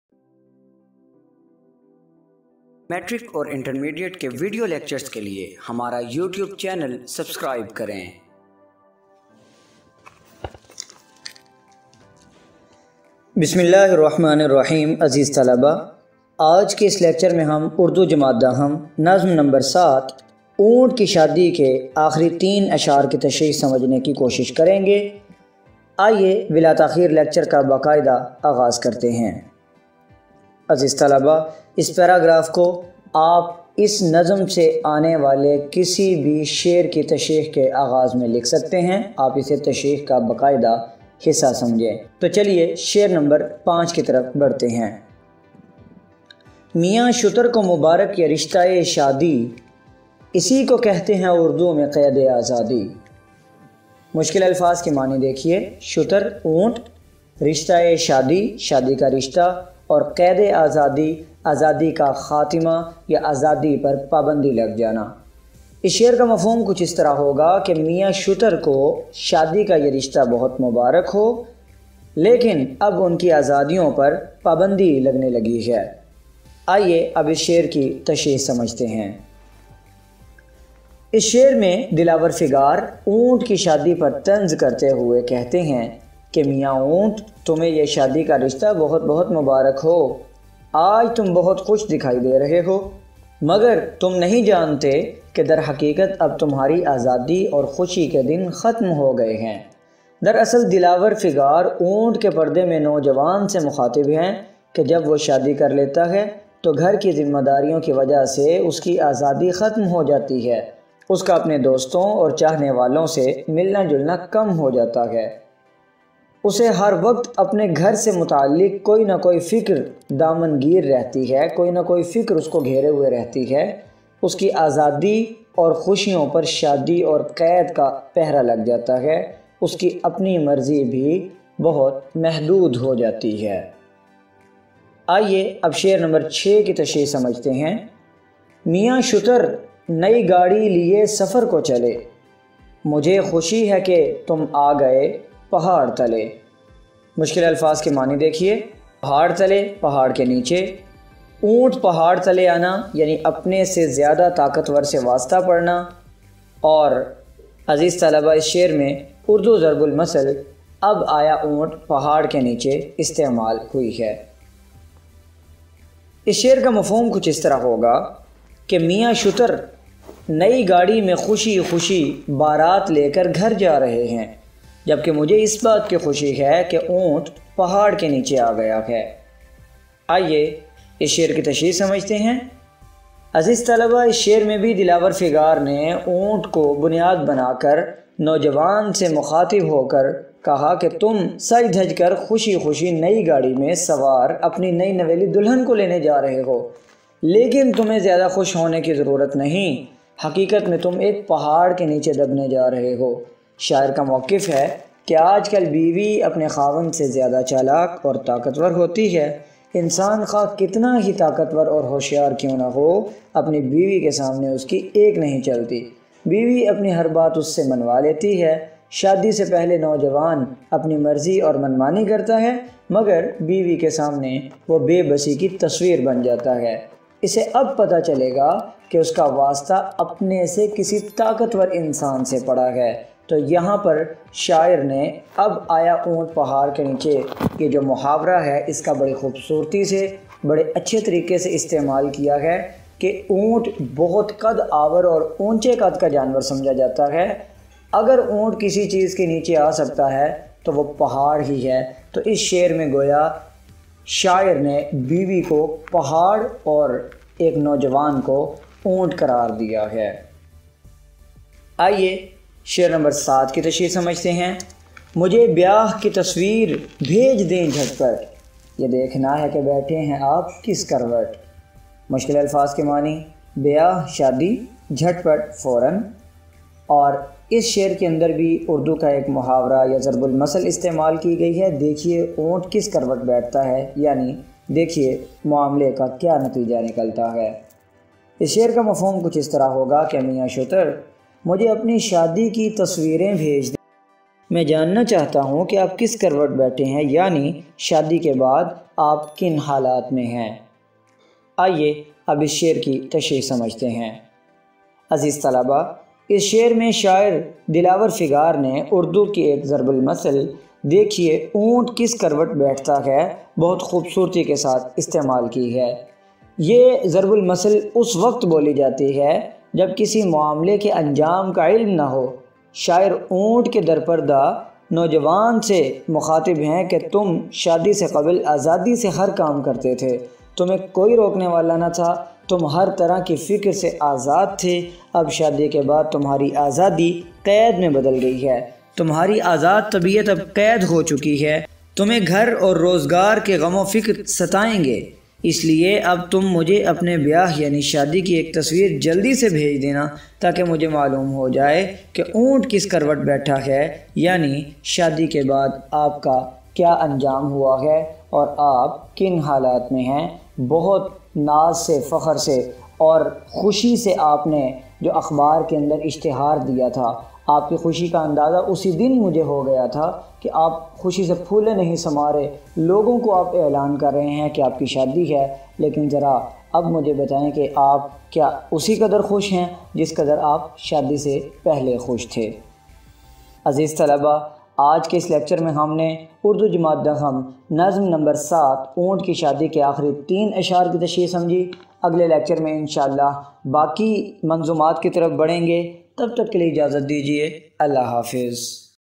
Nazm Oont Ki Shadi Tashreeh | Urdu Class 10th Nazm - 7|Part 3/3 | Shair No. 5, 6, 7
اونٹ کی شادی
نیا یہ آج کے پرچہ نے گل کھلایا ہے
کہ سہرا باندھ کے اک اونٹ بلبلایا ہے

شتر کے گھر میں پیامِ بہار ہے سہرا
کبھی کبھی تو بڑا بے مہار ہے سرا

مرے بنے کو مبارک یہ خوش گوار گھڑی
کہ سر کا درد بڑھا ناک میں نکیل پڑی

سمجھ لیا تھا جسے جانور سواری کا
وہ اونٹ بوجھ اٹھائے گا ذمہ داری کا

میاں شتر کو مبارک یہ رشتۂ شادی
اسی کو کہتے ہیں اردو میں قیدِ آزادی

میاں شتر نئی گاڑی کے سفر کو چلے
مجھے خوشی ہے کہ تم آ گئے پہاڑ کے تلے

مجھے بیاہ کی تصویر بھیج دیں جھٹ پٹ
یہ دیکھنا ہے کہ بیٹھے ہیں آپ کس کروٹ

شبِ وصال نہ کرنا   شتر  کوئی غمزہ
کہ ناروا ہے اضافت کے بعد اک ہمزہ

شاعر: دلاور فگار

In this Lecture, we will try to understand the Tashreeh/Explanation of Poem Oont Ki Shadi (written by Dilawar Figaar) of Class 10th (Punjab Text Book Board). This is the 3rd and last part of the Lecture out of 3 (Part 3/3) which is based on Stanza No. 5, 6, 7.

حصہ نثر مختصر سوالات کے جوابات

                                                                                                    By
                                                                                   Hafiz Abdul Mannan Aziz
                                                                                                M.Phil
I want to "Educate" students using digital means and helping students who want to study but can't afford the hefty fees.